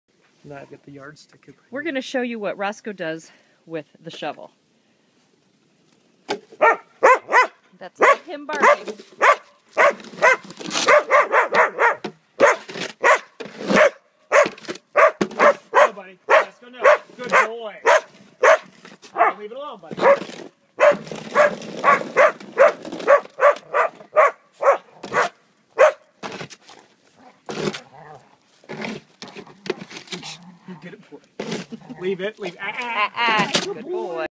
Roscoe & the shovel
Foster Malinois Roscoe is still learning to be nice to the shovel.  That's Luke barking in the background.